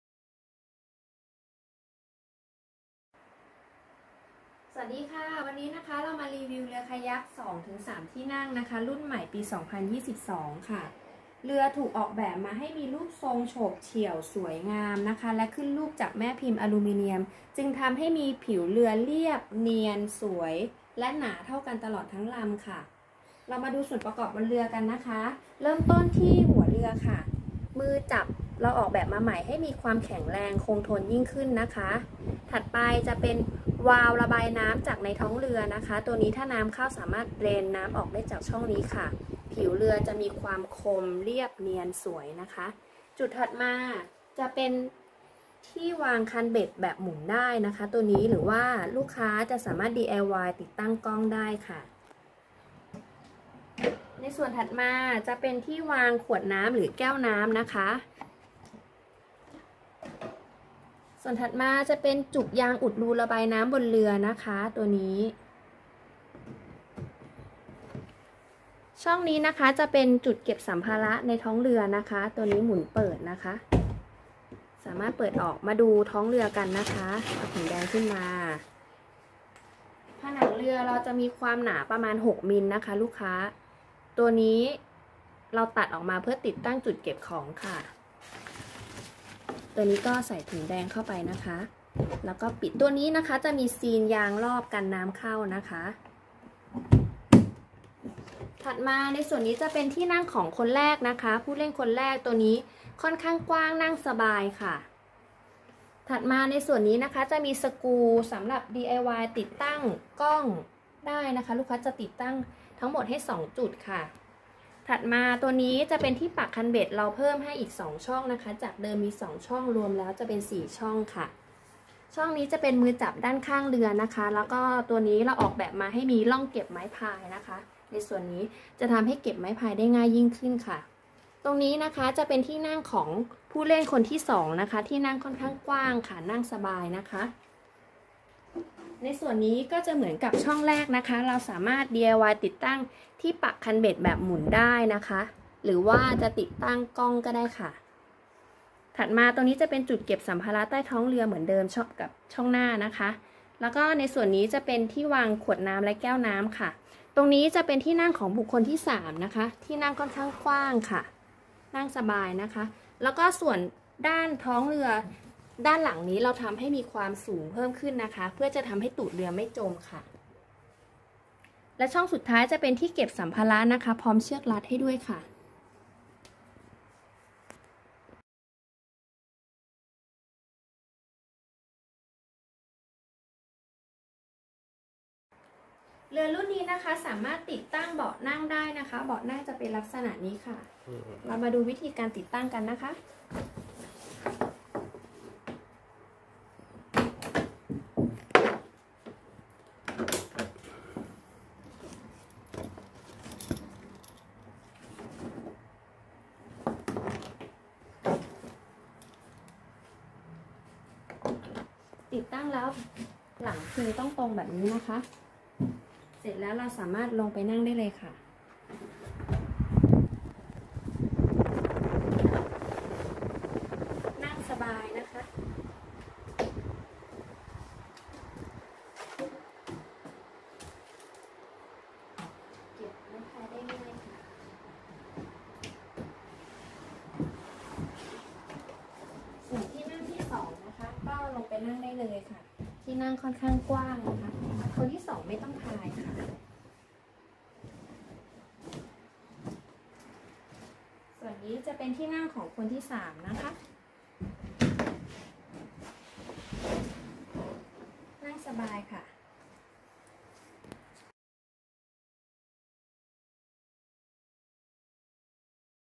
รีวิว คายัค 2-3 คนนั่ง รุ่นใหม่ 2023
คายัค 2-3 คนนั่ง รุ่นใหม่ ปี 2023
ถูกออกแบบมาให้มีรูปทรงโฉบเฉี่ยว สวยงาม และขึ้นรูปจากแม่พิมพ์อลูมิเนียม จึงทำให้ผิวเรือเรียบเนียน และมีความหนาเท่ากันทั่วทั้งลำ

มือจับหัว-ท้ายเรือ เปลี่ยนใหม่ให้มีความแข็งแรงขึ้น ที่นั่งกว้าง นั่งสบาย มีร่องเก็บไม้พาย 2 ข้าง และเพิ่มที่ปักคันเบ็ดให้อีก 2 ช่อง

ท้ายเรือออกแบบมาให้สูงขึ้น ทำให้ตูดเรือไม่จมน้ำ สามารถใช้งานได้ทั้งน้ำเรียบ และมีคลื่น